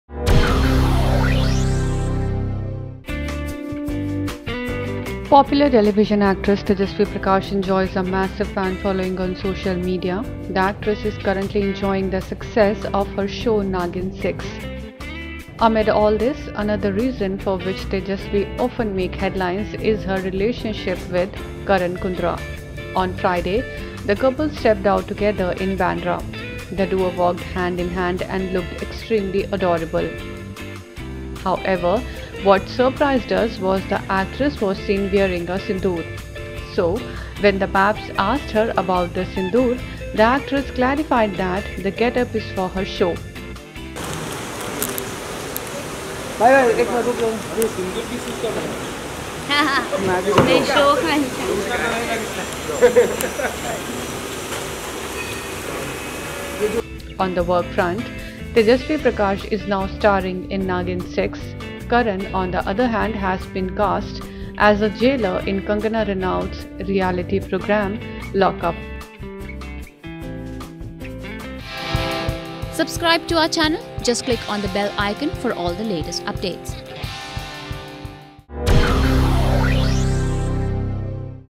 Has Tejaswwi Prakash tied the knot with beau Karan Kundrra?
Popular television actress Tejaswwi Prakash enjoys a massive fan following on social media. The actress is currently enjoying the success of her show 'Naagin 6'.